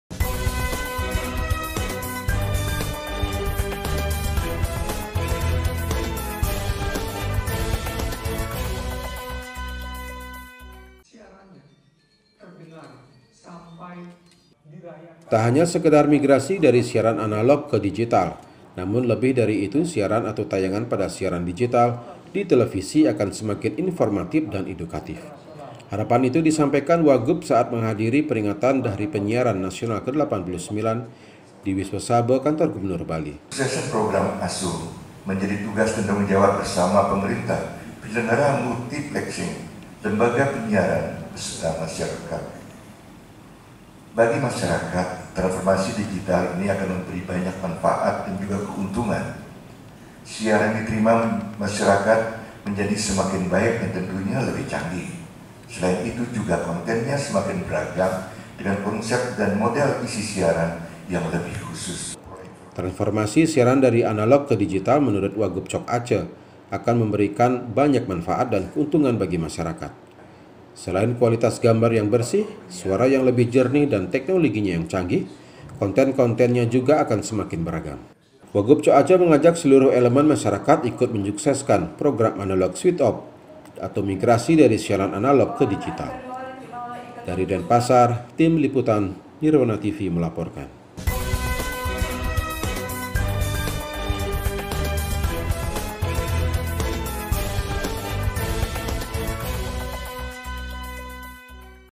WAGUB COK ACE INGIN TAYANGAN SIARAN DIGITAL INFORMATIF DAN EDUKATIF | NIRWANA UPDATE
TAK HANYA SEKEDAR MIGRASI DARI SIARAN ANALOG KE DIGITAL, NAMUN LEBIH DARI ITU SIARAN ATAU TAYANGAN PADA SIARAN DIGITAL DI TELEVISI AKAN SEMAKIN INFORMATIF DAN EDUKATIF. HARAPAN ITU DISAMPAIKAN WAGUB SAAT MENGHADIRI PERINGATAN HARI PENYIARAN NASIONAL KE-89 DI WISWA SABHA KANTOR GUBERNUR BALI. TRANSFORMASI SIARAN DARI ANALOG KE DIGITAL MENURUT WAGUB COK ACE, AKAN MEMBERI BANYAK MANFAAT DAN KEUNTUNGAN BAGI MASYARAKAT. SELAIN KUALITAS GAMBAR YANG BERSIH, SUARA YANG LEBIH JERNIH, DAN TEKNOLOGINYA CANGGIH, KONTEN-KONTENNYA JUGA AKAN SEMAKIN BERAGAM.